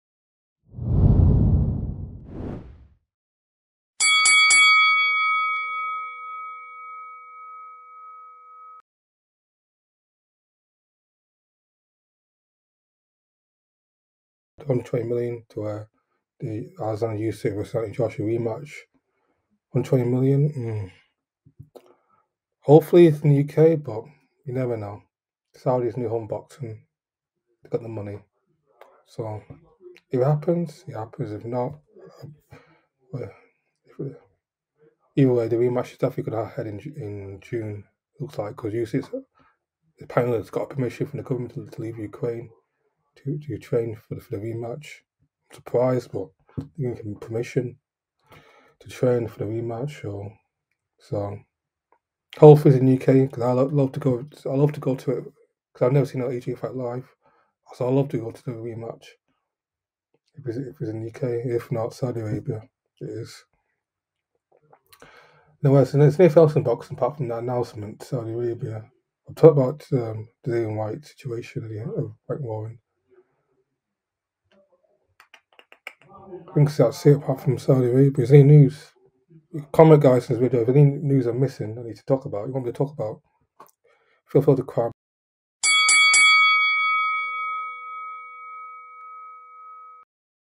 Usyk/Joshua rematch in Saudi Arabia?
120 Million has apparently been offered for the Usyk/Joshua rematch to be held in Saudi Arabia.